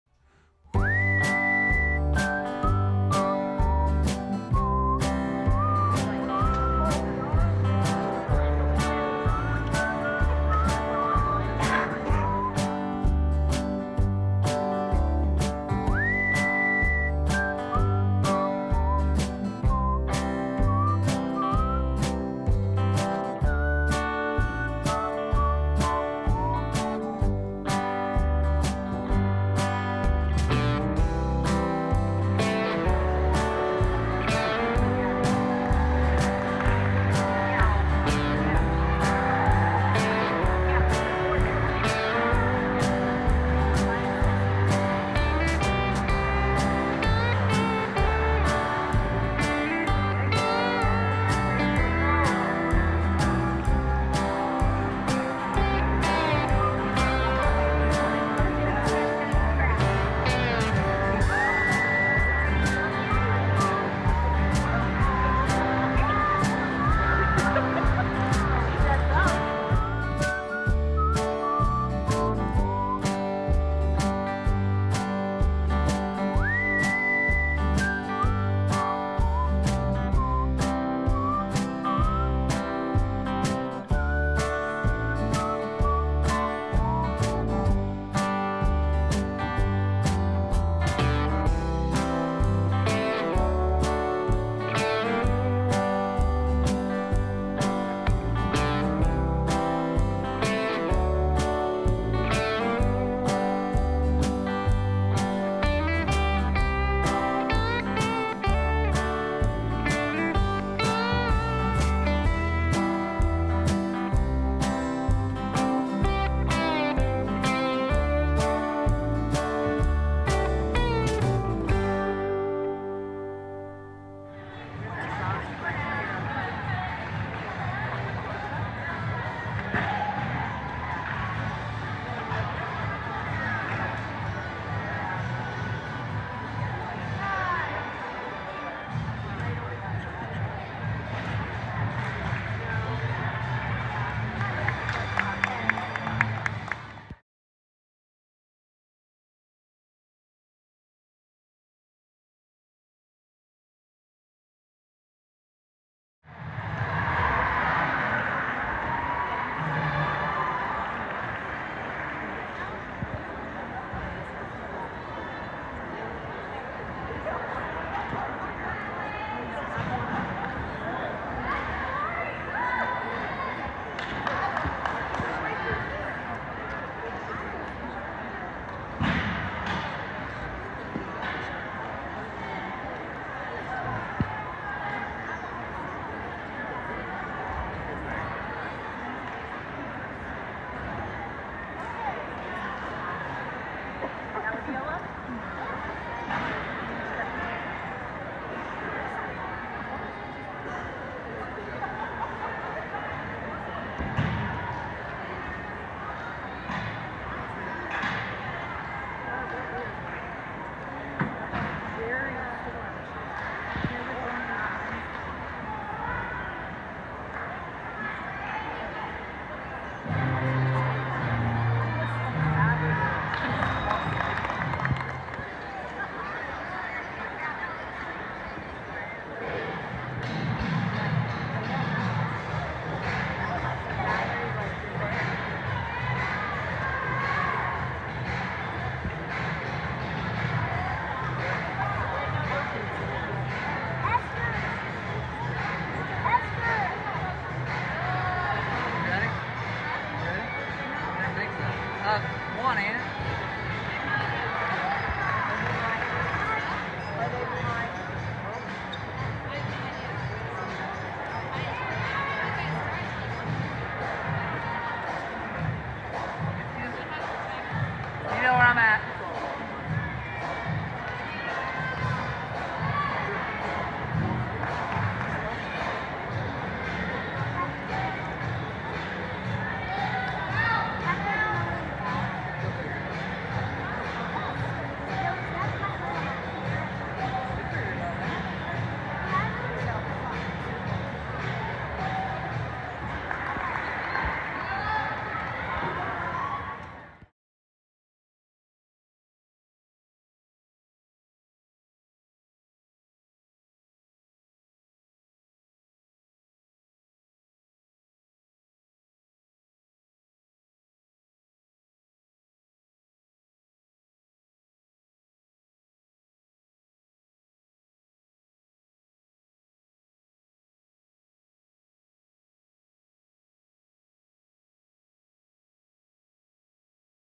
Blaine Wilson Sportsfest in Columbus, OH. 2-15-2020
Emily and I took a road trip to Columbus, OH where she competed in the Blain Wilson Sportsfest at the Ohio Expo Center. She had an awesome meet getting 2nd on vault,  2nd on bars, 5th on beam, 1st on floor and 2nd All around.  And of course, we had to stop at Chic-fil-a!  February 15, 2020